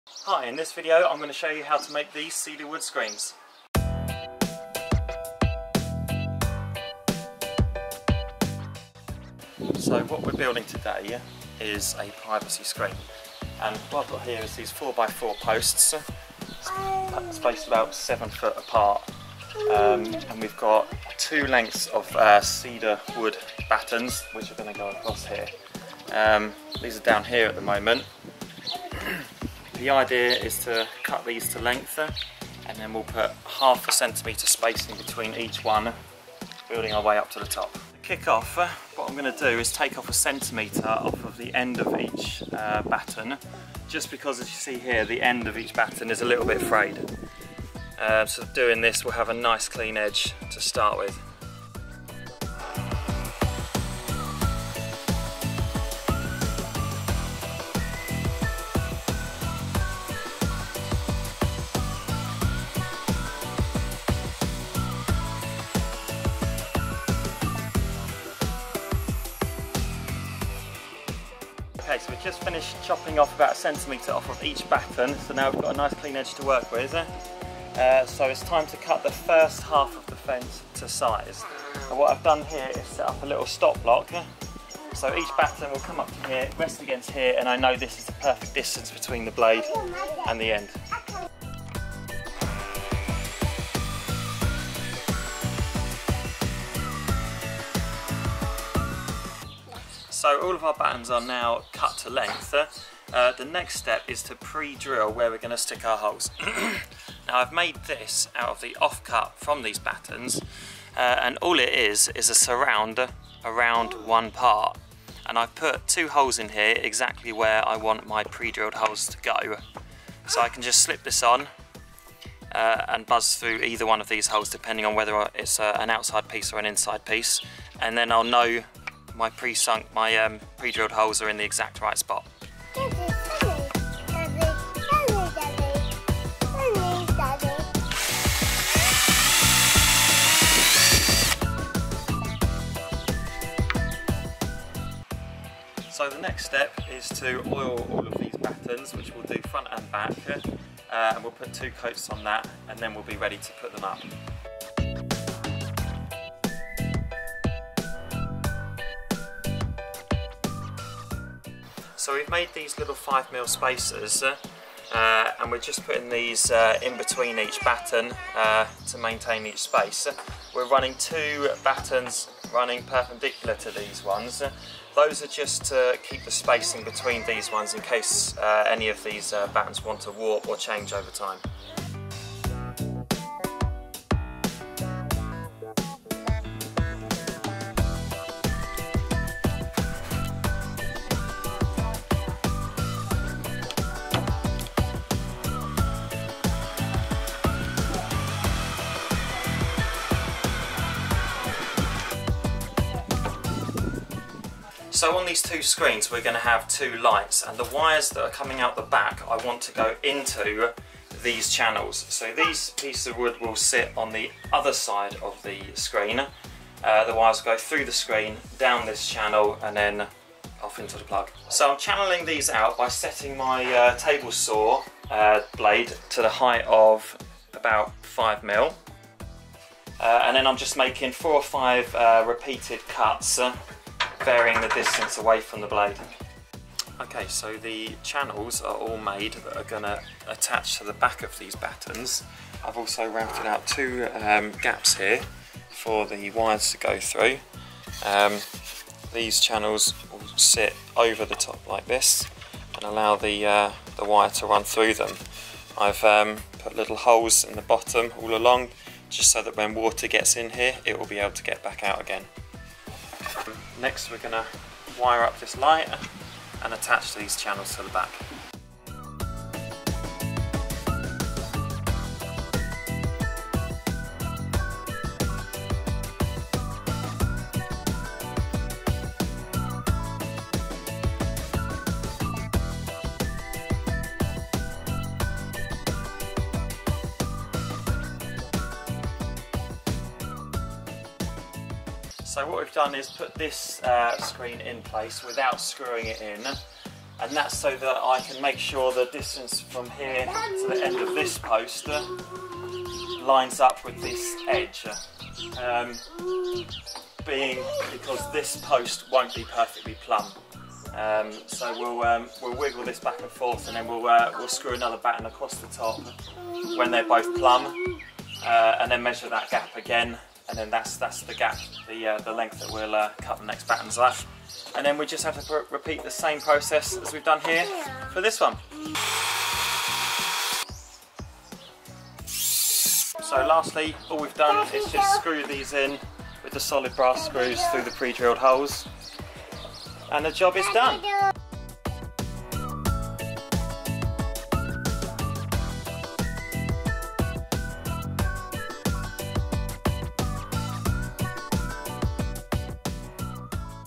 How to make DIY cedar fence panels with built in lights | The DIY Tribe
In this video we show you how we made our horizontal fence panels / privacy screen. The fence panels are made from western red cedar battens, mounted to 4x4 posts.

:: Materials and tools ::

Brass screws - Amazon
Oil - Amazon
Chop saw - Amazon
Table saw -Amazon
Combi drill - Amazon